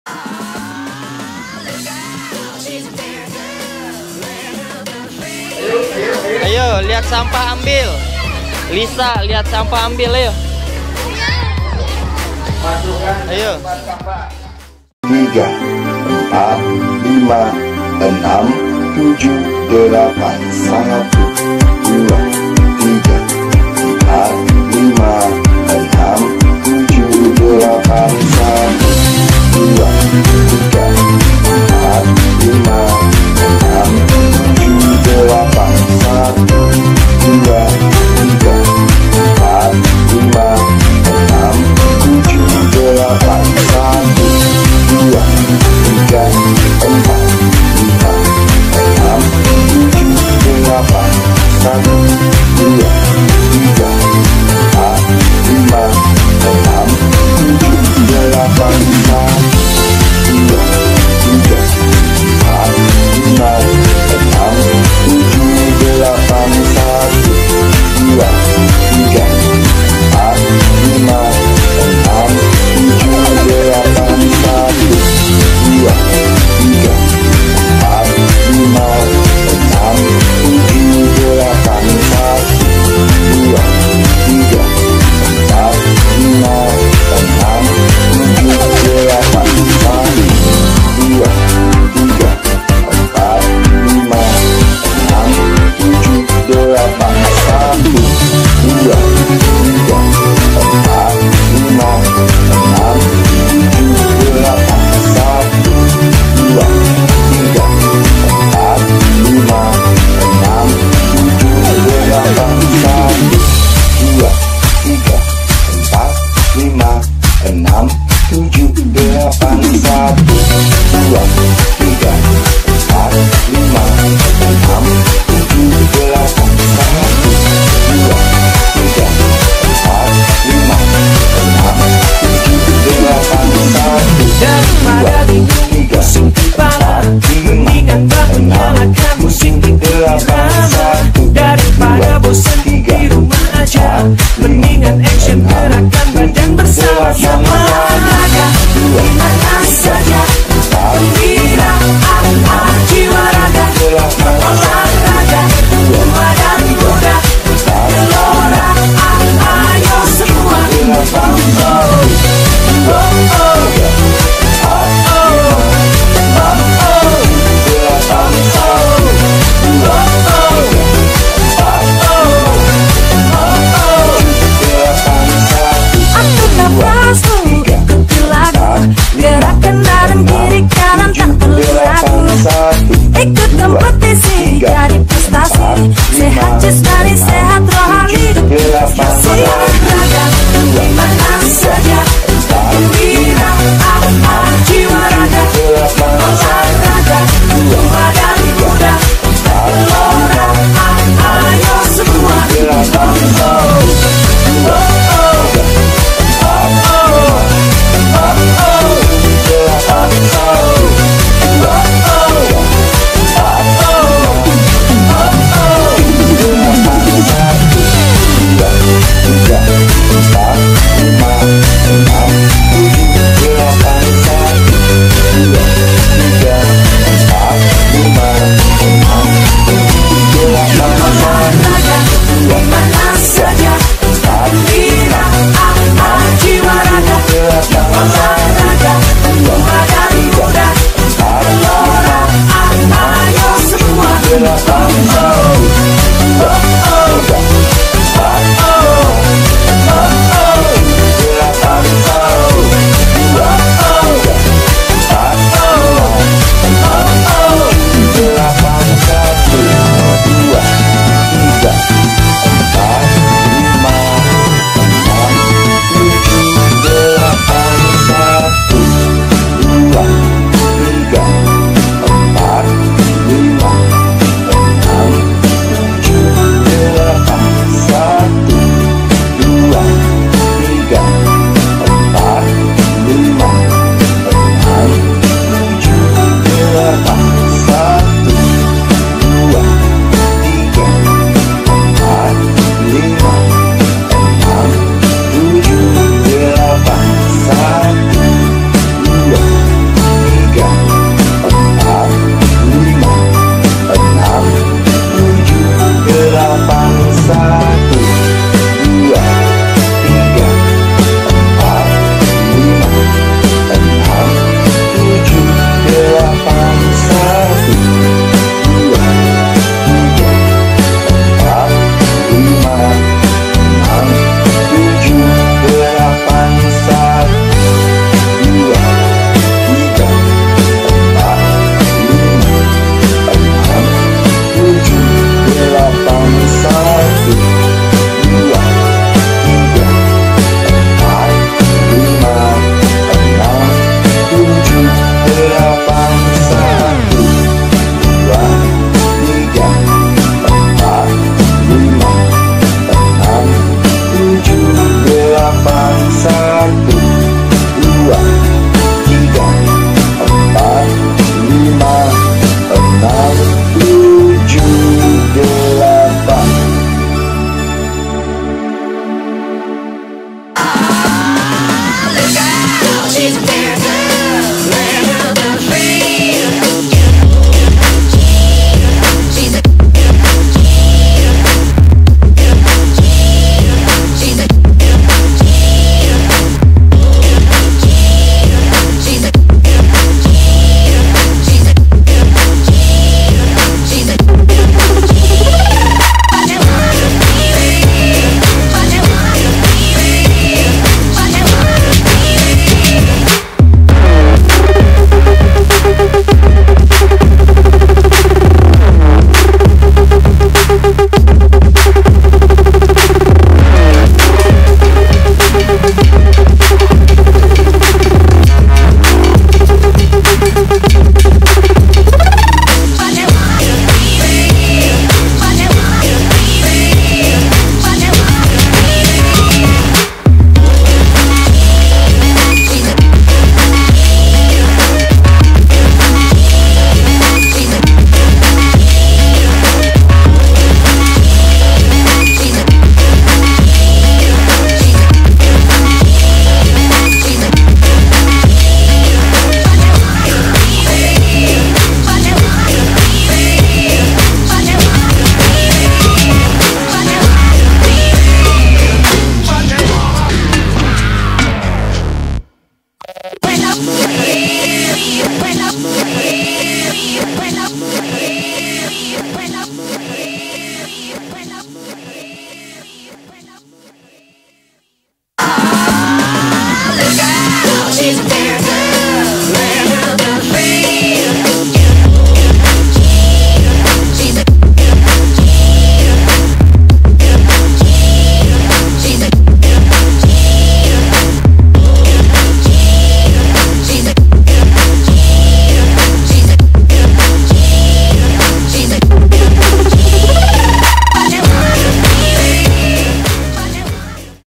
🔴SenamSKJPelajar2023Terbaru|SdnEkasariPamanukan|Kegiatan PembiasaanHariSabtu|senamdanbebersihsekolah
KEGIATAN PEMBIASAAN SDN EKASARI SETIAP HARI SABTU
SENAM SKJ PELAJAR & OPERASI KEGIATAN MEMBERSIHKAN LINGKUNGAN SEKOLAH